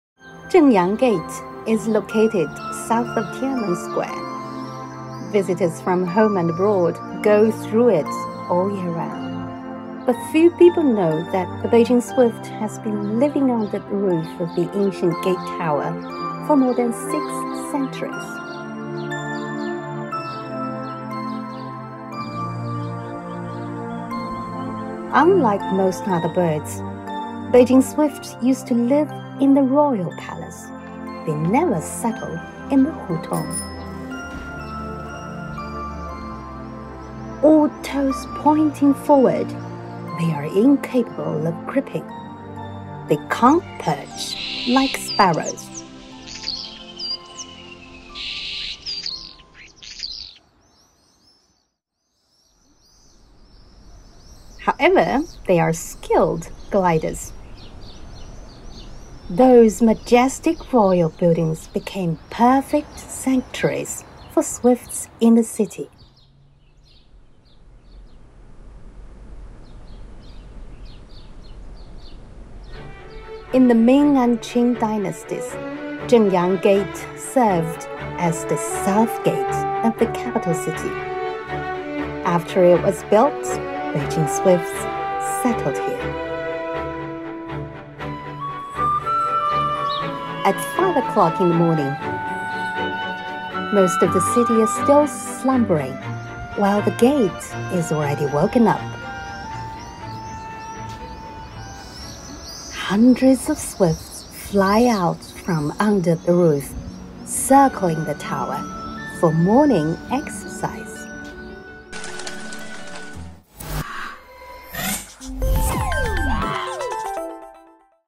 Summer in Beijing Series: Beijing swift
Zhengyang Gate (正阳门) is located south of TiananmenSquare, visitors from home and abroad go through it all year round. But few people know that the Beijing swift (雨燕 yǔ yàn) is also one of the regular visitors to this particular place for over six hundred years.